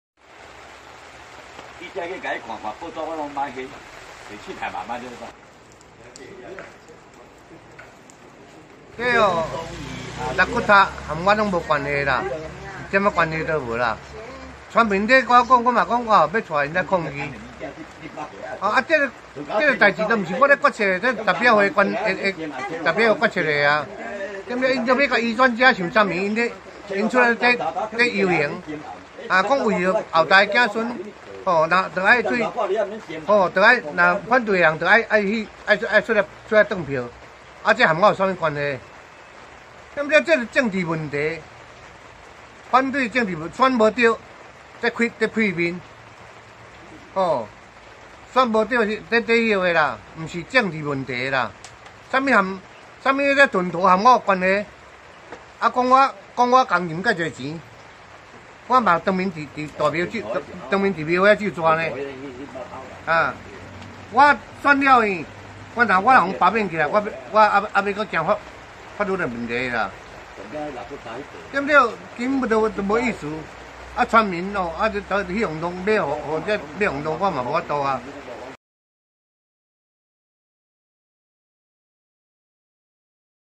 屏東縣南州鄉壽元村長罷免案今天投票 兩派人馬五五波
屏東縣南州鄉壽元村長罷免案今天投票，全村為1033票，必須超過259票同意才罷免成立，現任村長陳明倫表示，納骨塔的設立是鄉公所，不是村長能夠決定，認為這次的罷免案是政治的操作，由於地方選舉票數差距都很接近，新制規定1/4就能罷免，將造成很多的困擾。